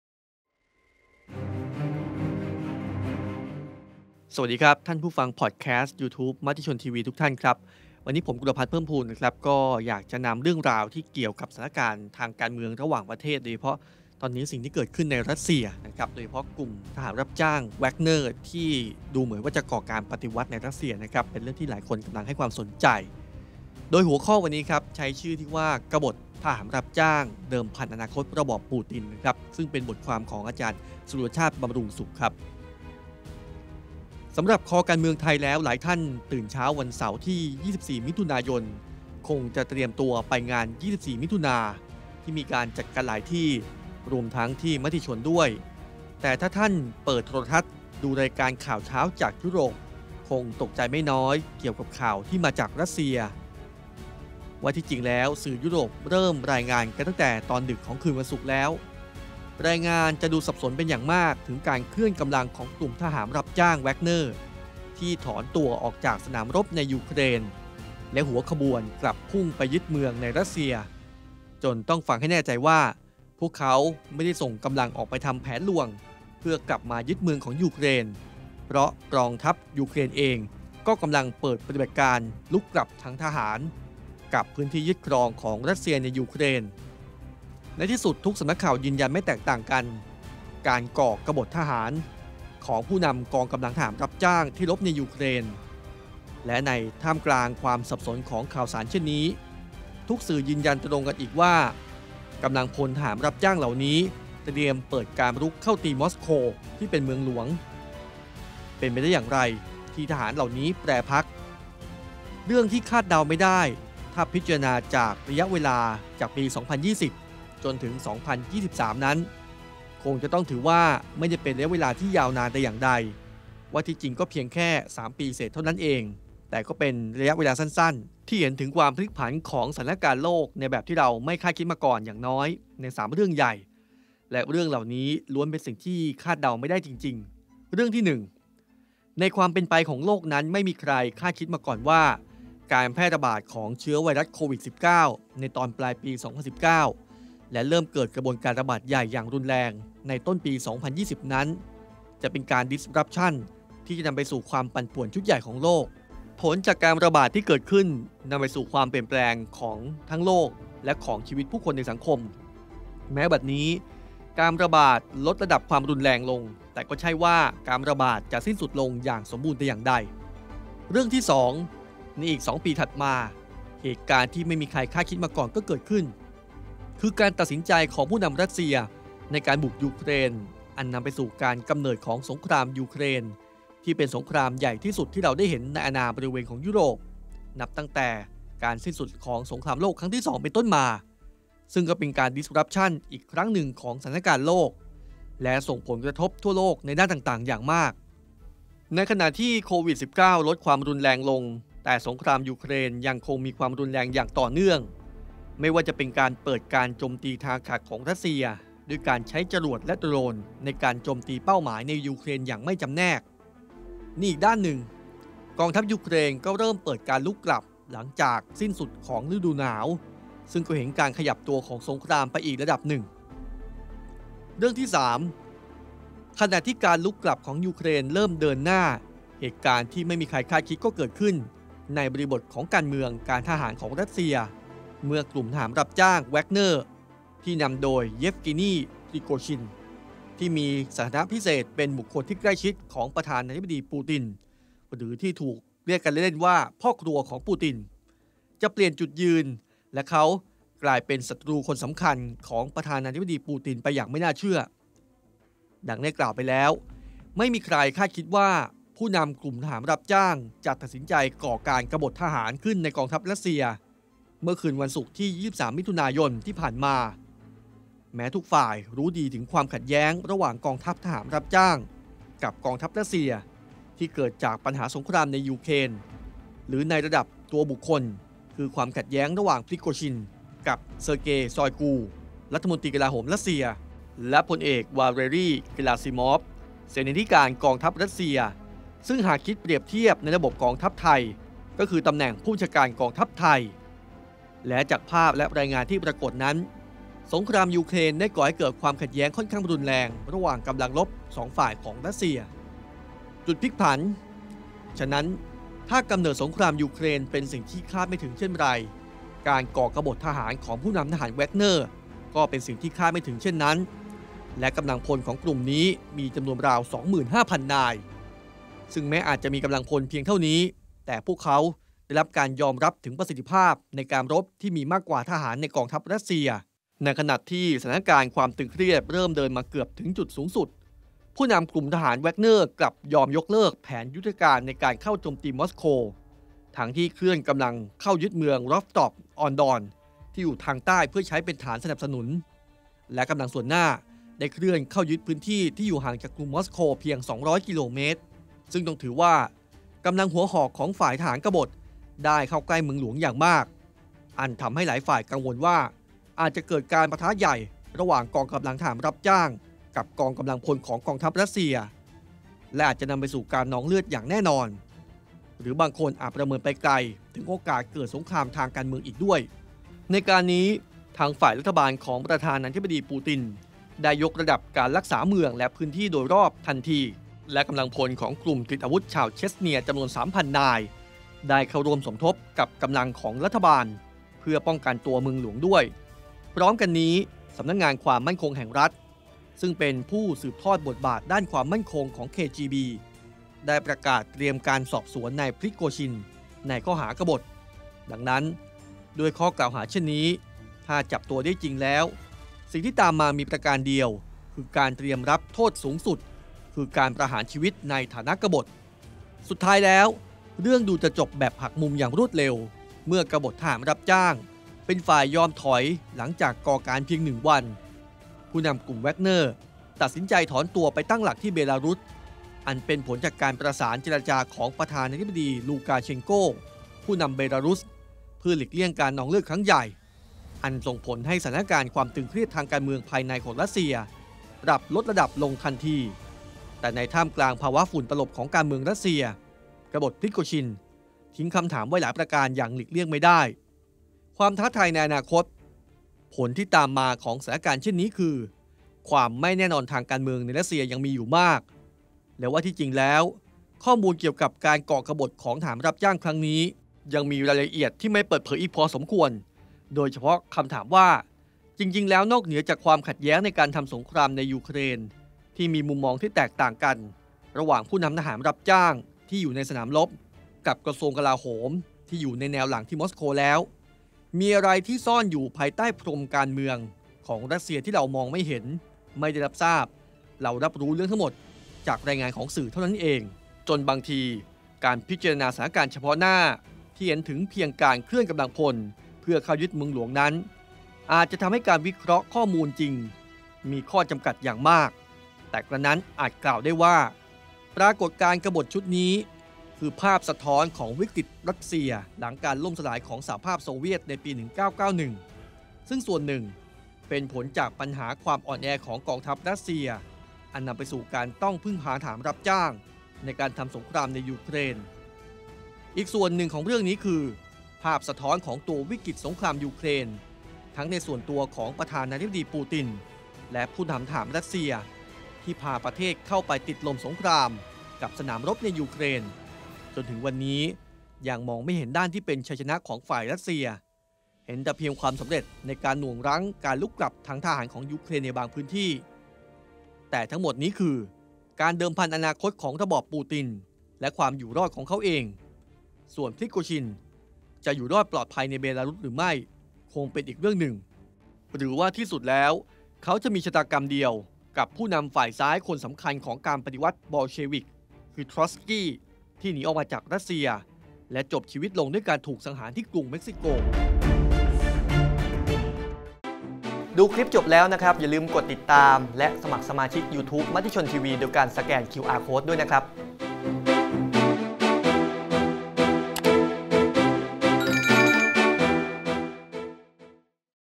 Podcast#การเมือง :กบฏทหารรับจ้าง! เดิมพันอนาคต ระบอบปูติน ศ.ดร.สุรชาติ บำรุงสุข: Matichon TV
Podcast#การเมือง : กบฏทหารรับจ้าง! เดิมพันอนาคต ระบอบปูติน ศ.ดร.สุรชาติ บำรุงสุข  ท่ามกลางภาวะ “ฝุ่นตลบ”  การเมืองรัสเซีย “กบฏพริโกซิน”  ทิ้งคำถามไว้หลายประการอย่างหลีกเลี่ยงไม่ได้